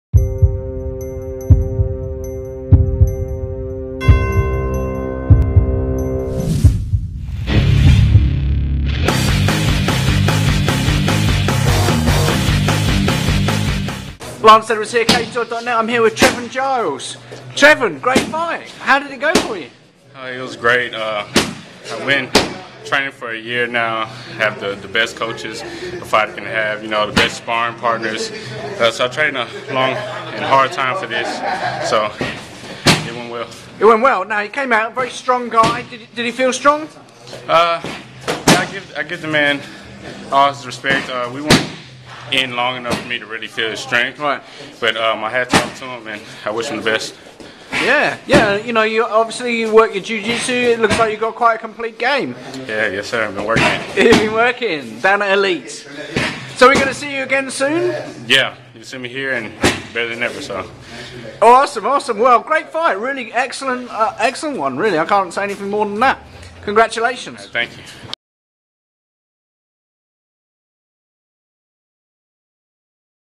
Cage Combat 7 Post Fight Interview with Trevin Giles
TheCageDoor.net's post fight interview with Trevin Giles at Garcia Promotions - Cage Combat 7.